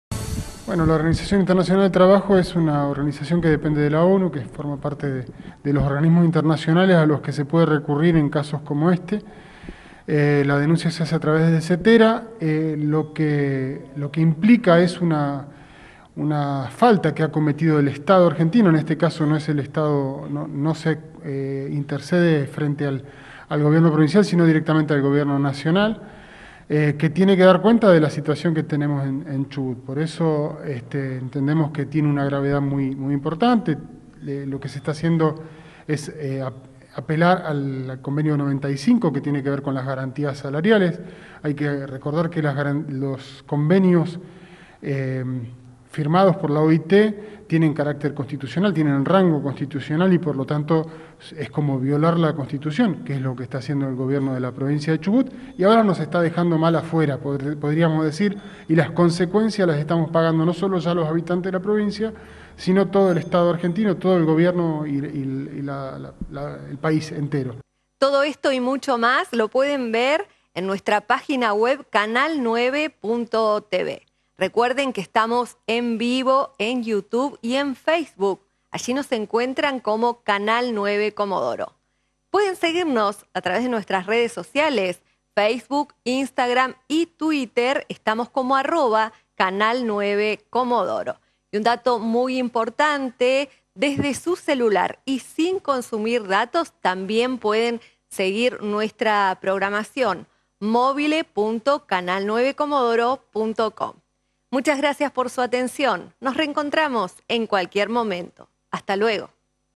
Atech denuncia ante la OIT incumplimiento del Gobierno
La Regional Sur de ATECh anunció que junto a CTERA presentaron una denuncia ante la Organización Internacional del Trabajo (OIT) por los atrasos en los pagos de salarios. Al respecto desde ATECh señalaron que “el estado argentino fue denunciado internacionalmente por los incumplimientos del gobierno de Arcioni”.